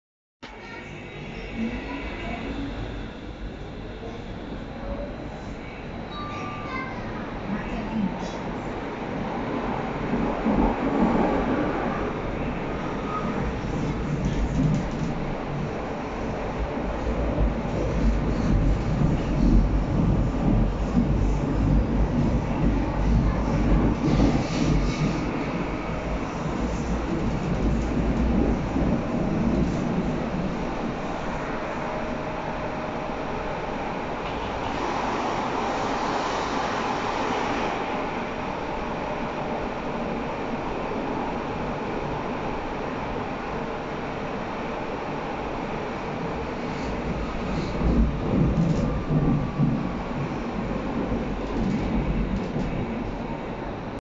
BVG Berlin U Bahn mitfahrt IK Zug Linie U5 Berlin Lichtenberg
Zug nach Alexanderplatz